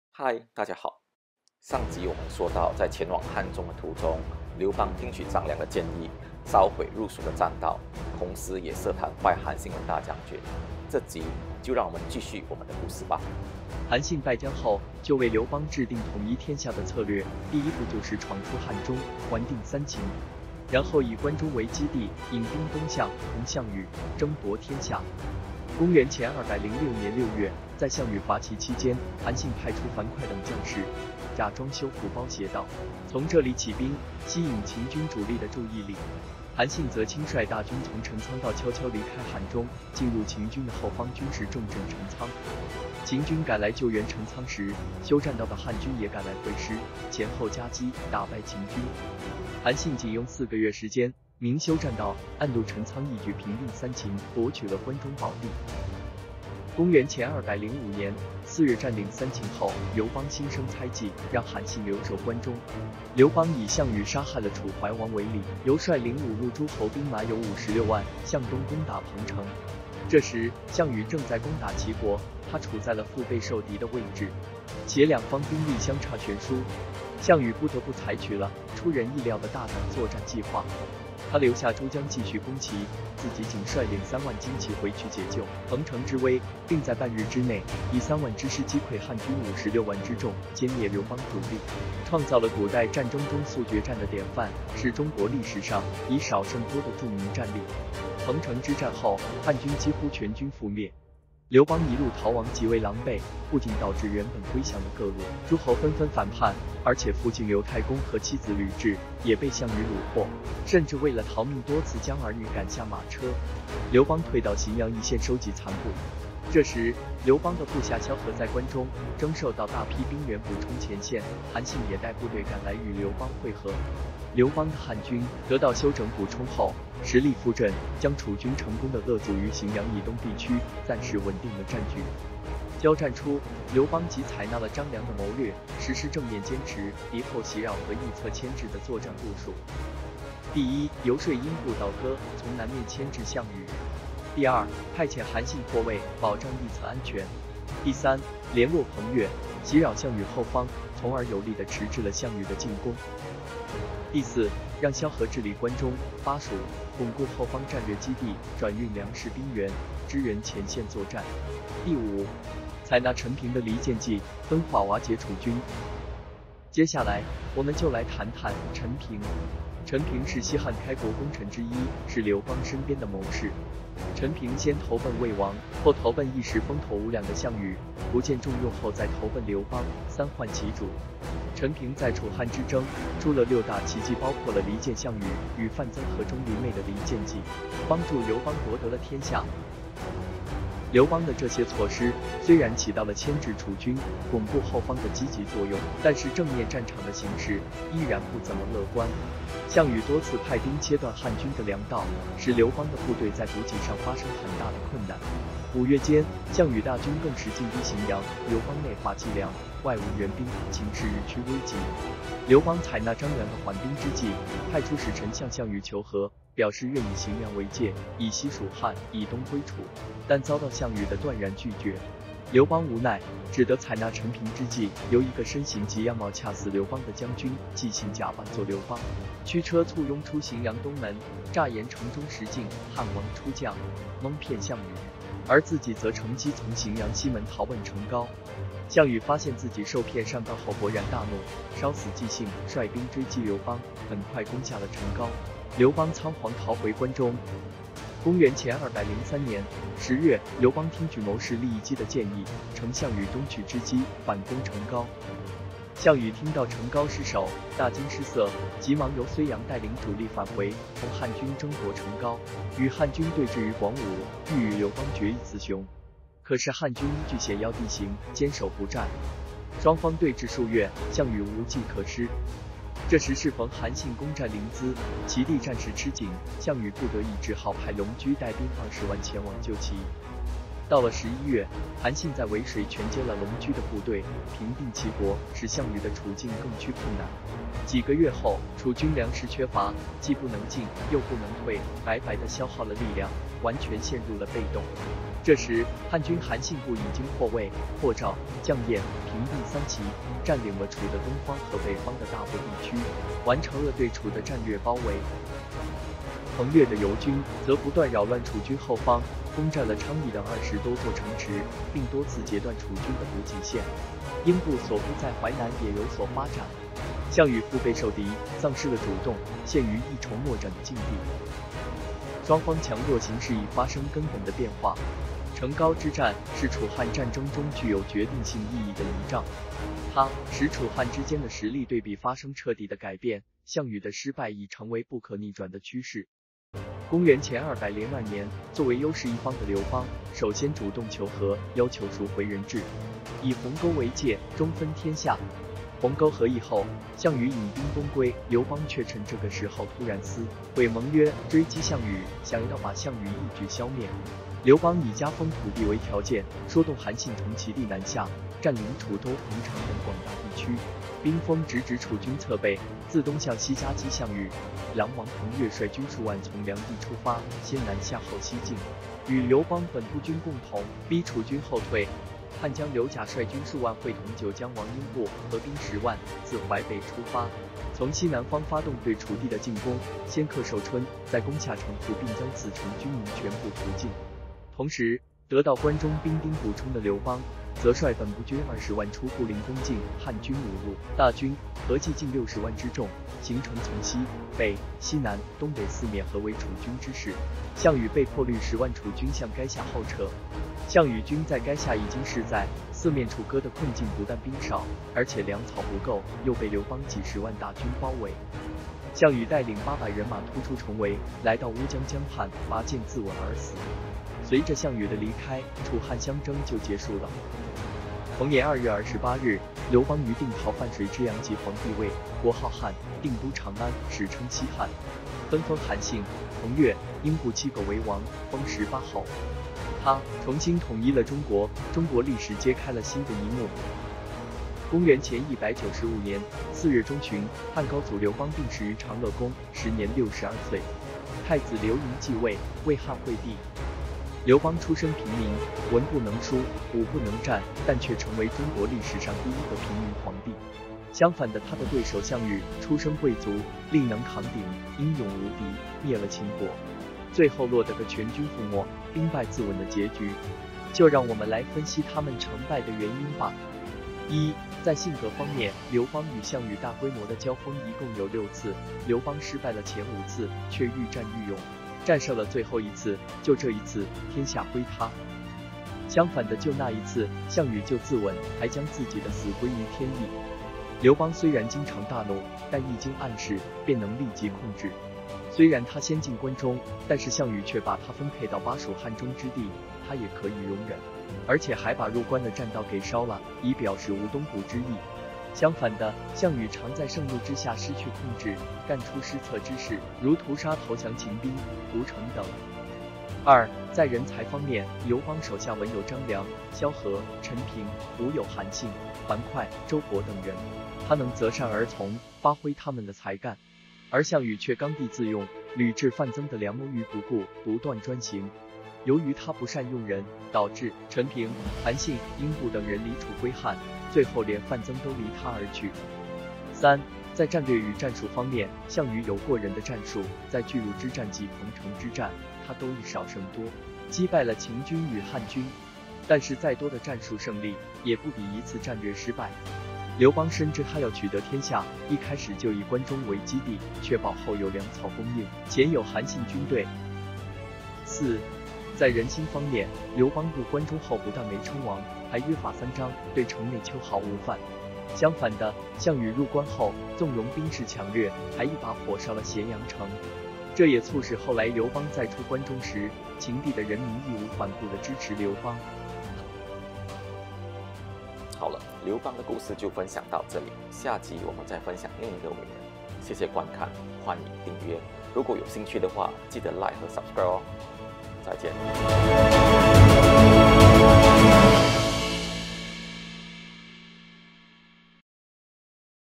十分钟看刘邦如何逆袭成功成为平民皇帝
看了陈道明与何润东主演的连续剧"楚汉传奇"，而对这刘邦这个人特别感兴趣。刘邦仅仅用了7年时间便从泗水亭长摇身一变，成为了汉朝的开国皇帝。更让人不得不佩服的是他"摇身"的时候已经是38岁了，他的对手则是个有背景，有能力，几乎样样都比他强的项羽。看了些书，收集了些资料，做了这个短片与大家分享。